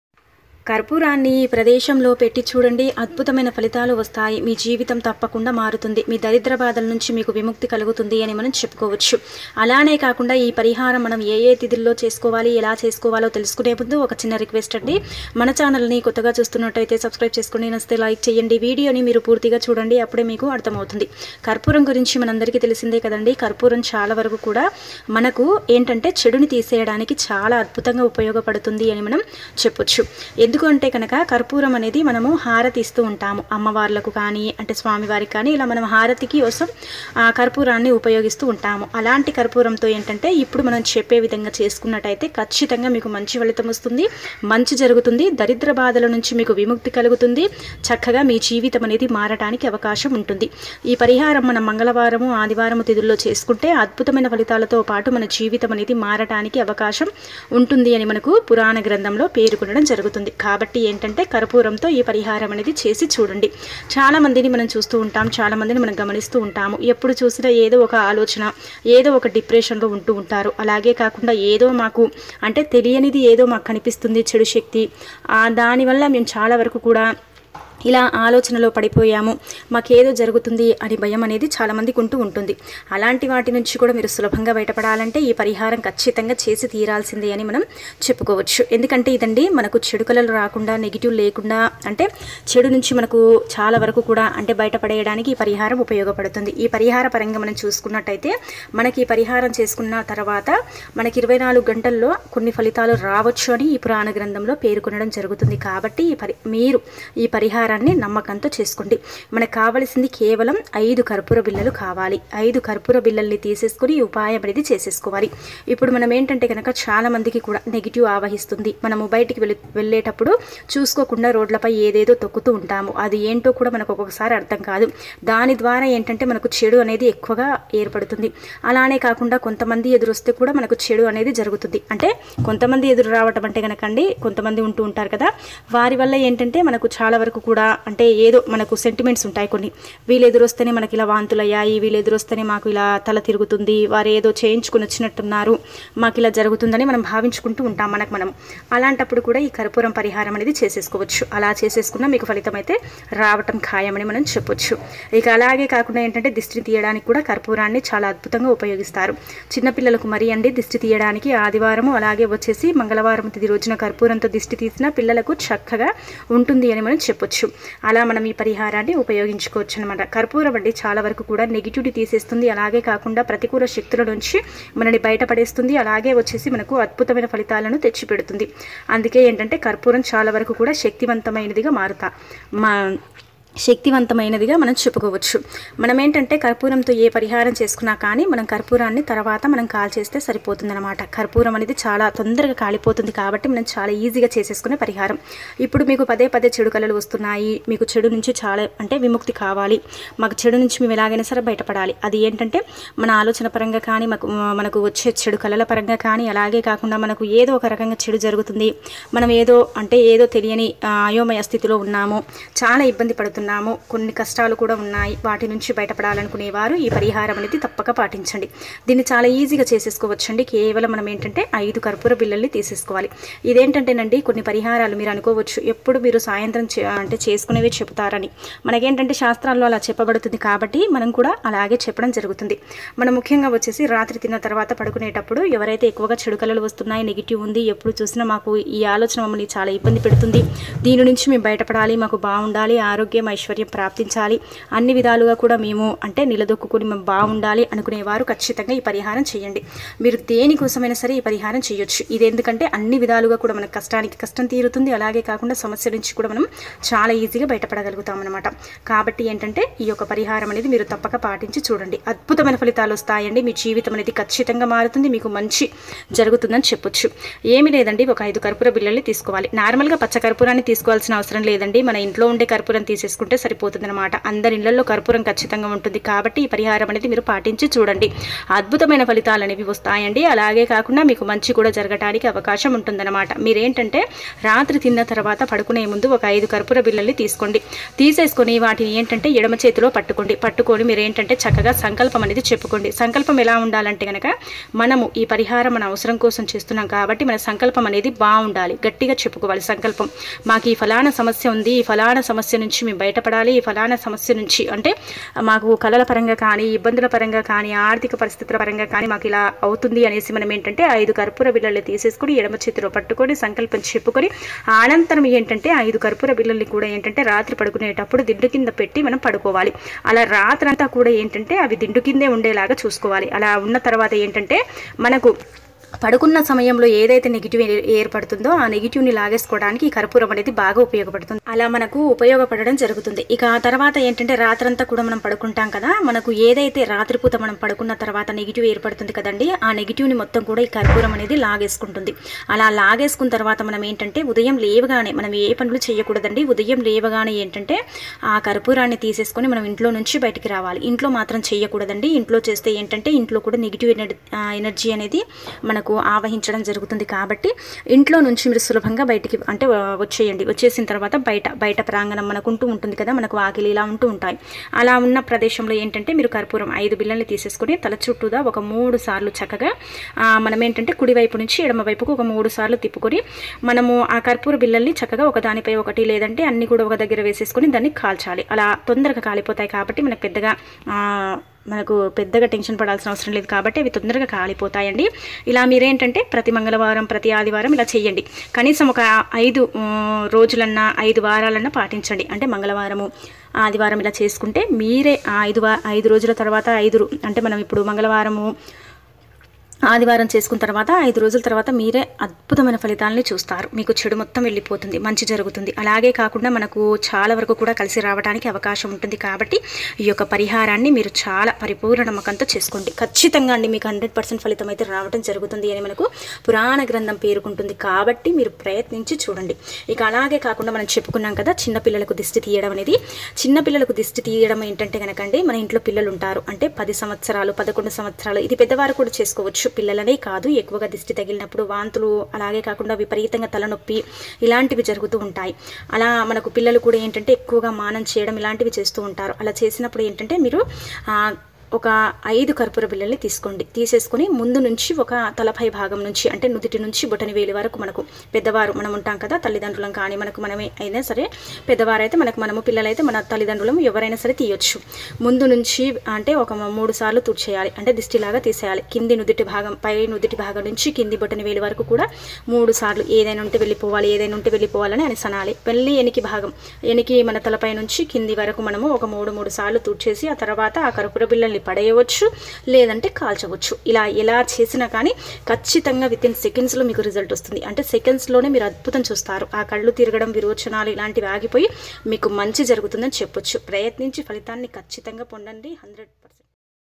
రేపు ఆదివారం రోజు రాత్రి పడుకునే ముందు కర్పూరం ఈ ప్రదేశంలో పెడితే చాలు తెల్లవారేసరికి దరిద్రం పోయి..
రేపు ఆదివారం రోజు రాత్రి పడుకునే ముందు కర్పూరం ఈ ప్రదేశంలో పెడితే చాలు తెల్లవారేసరికి దరిద్రం పోయి డబ్బే డబ్బు